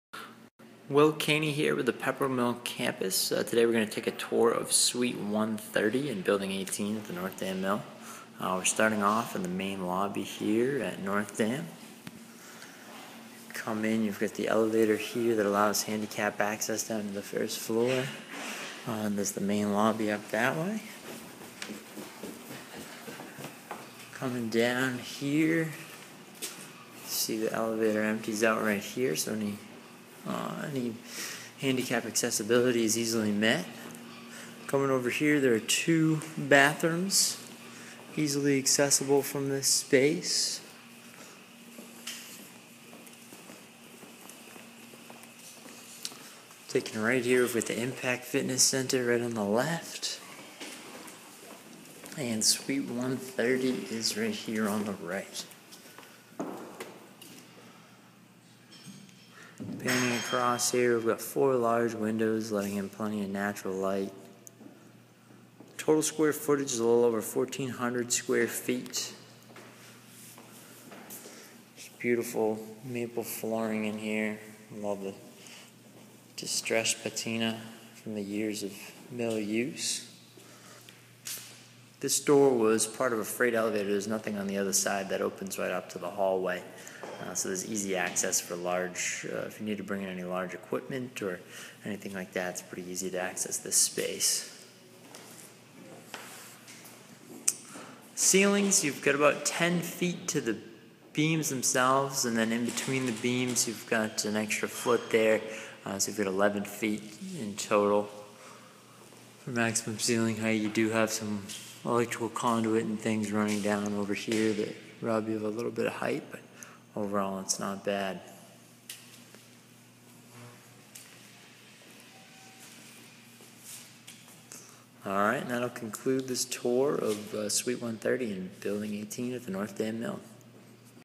Video Tour of Suite 130 in Building 18 at the North Dam Mill
Quick tour of suite 130 in building 18 at the North Dam Mill. The monthly rent includes, heat, electricity, internet, and parking.